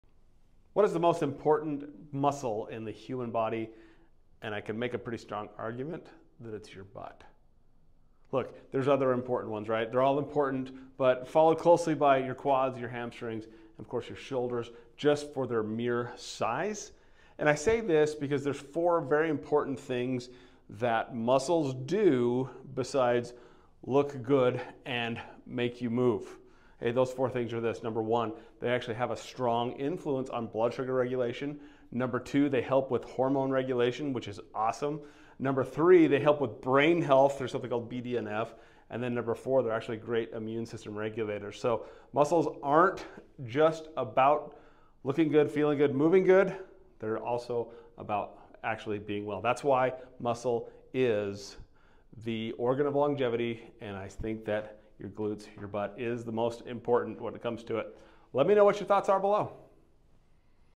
Whats the most important muscle in your body?
💥 What’s the most important muscle in your body?

Hint: It’s not your heart, abs, or even your brain… it’s your butt. Followed closely by your quads, hamstrings, and shoulders.

And it’s not about aesthetics or strength – it’s about:
✅ Blood sugar regulation – these big muscles act as glucose sinks, helping prevent insulin resistance.
✅ Hormone balance – muscle mass supports healthy testosterone, estrogen, and growth hormone levels.
✅ Brain health (BDNF) – training these muscles boosts brain-derived neurotrophic factor, protecting memory and focus.
✅ Immune health – activating these muscles releases anti-inflammatory myokines that strengthen your immune system.

🔥 If you want better health from the inside out, don’t skip leg day – and never ignore your glutes.